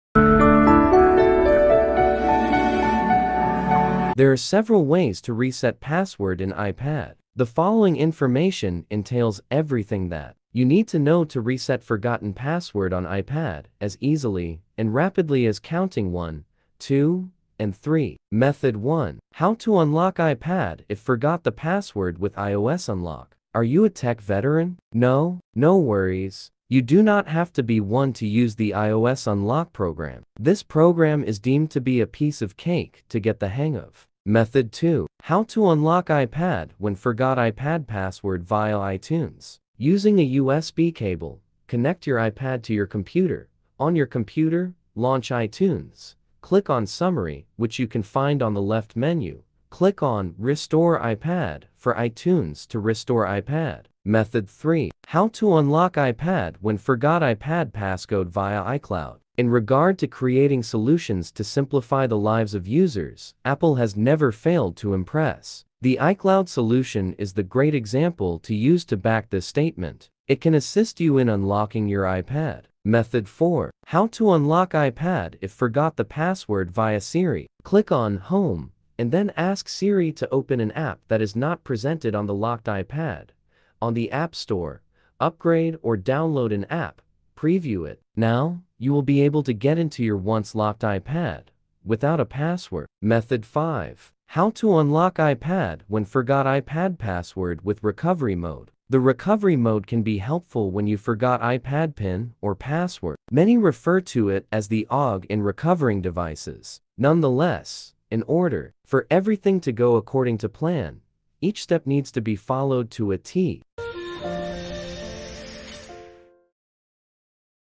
Forgot iPad Password? 5 Effective Methods Have Your Back
Forgetting iPad password is more common than what you may think. No password? No problem! There are seven effective ways to unlock your iPad Pro/Air/4/mini/2, so as to putting your iPad to good use again.